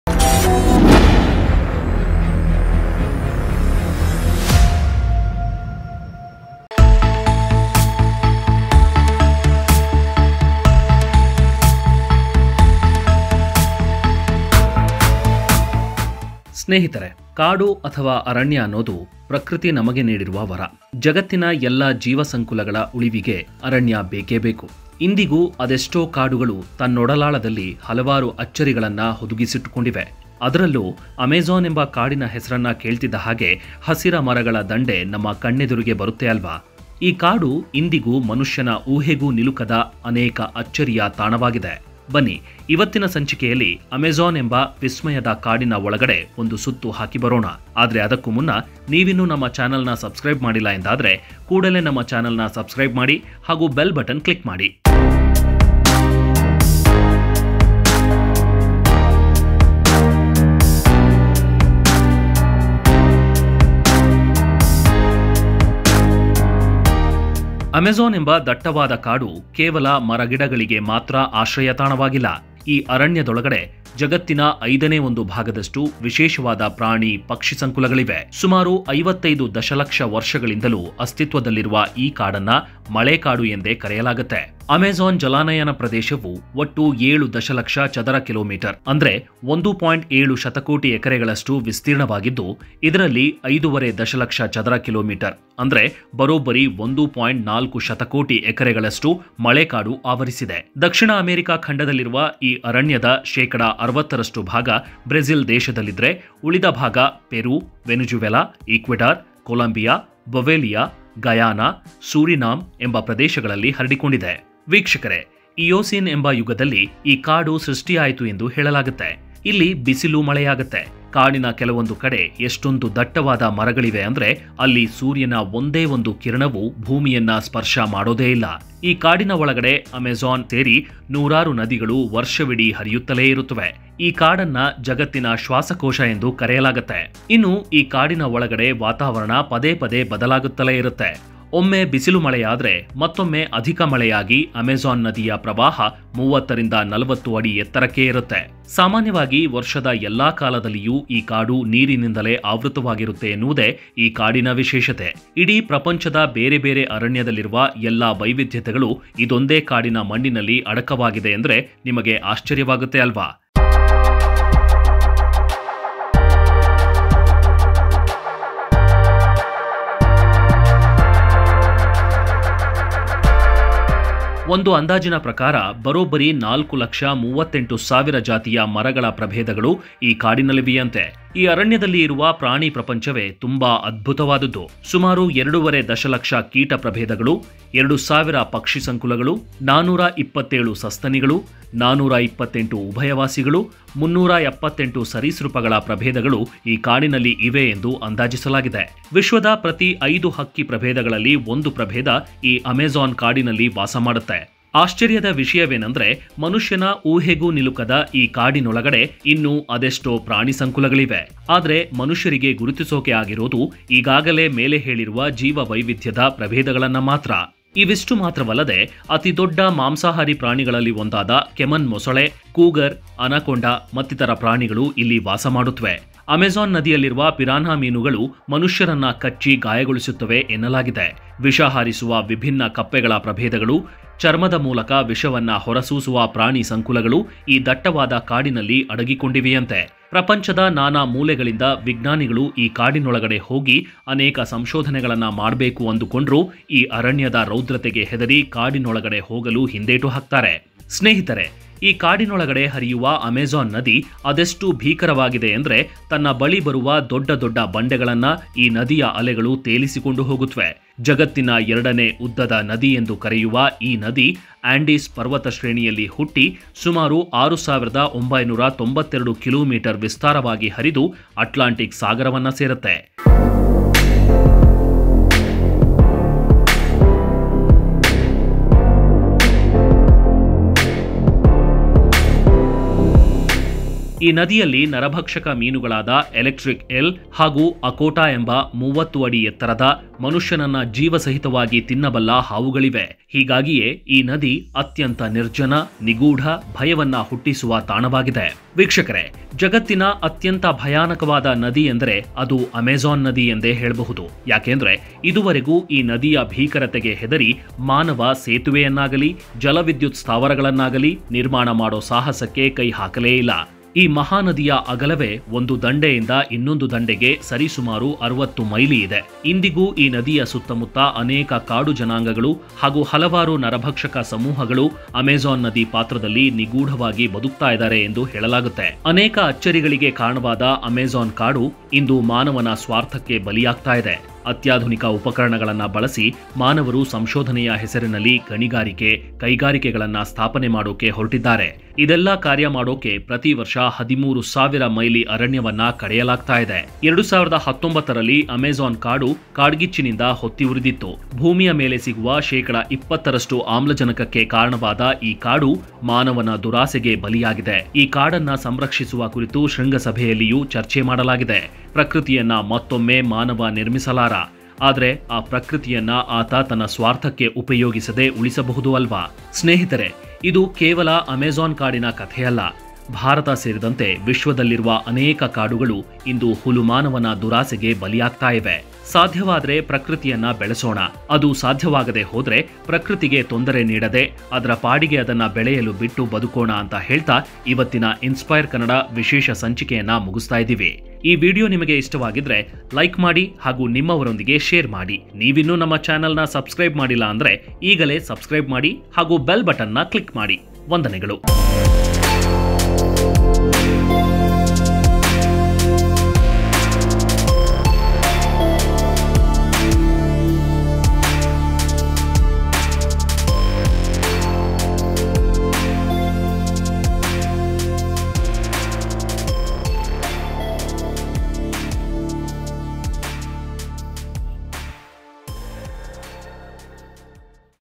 ಅಮೆಜಾನ್ - ಈ ಕಾಡು ಎಷ್ಟು ಭಯಾನಕವಾಗಿದೆ ಗೊತ್ತಾ..? | Amazon Forest | Inspire Kannada Official
Interesting facts on Amazon Rain forest in Kannada | Inspire Kannada Official